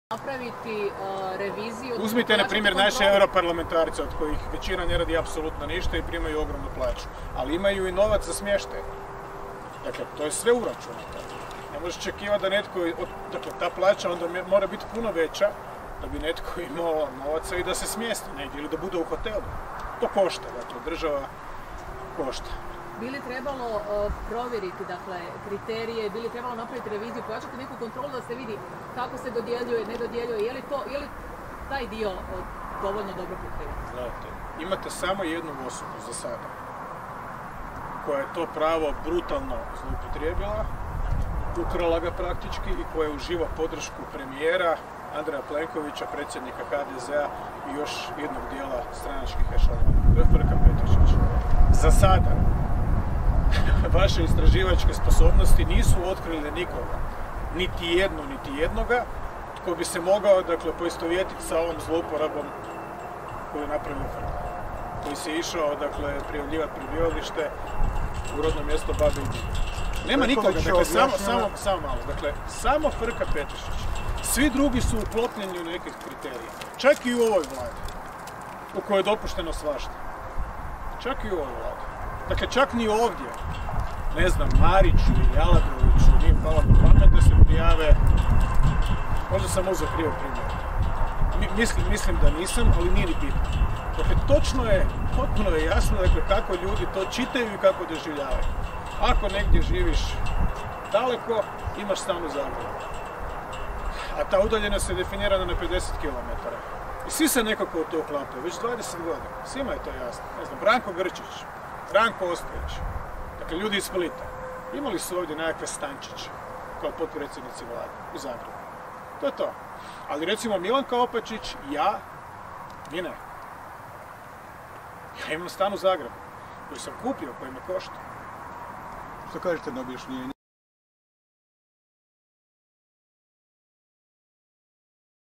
Predsjednik Milanović o Frki Petešiću i njegovoj zlouporabi
Predsjednik Republike Hrvatske Zoran Milanović posjetio Ustanovu za sveobuhvatnu skrb Tigrovi u Rakitju, te održao sastanak s predstavnicima Ustanove i vodstvom Udruge Tigar 90/91 Rakitje. Nakon sastanka predsjednik Zoran Milanović dao izjavu za medije.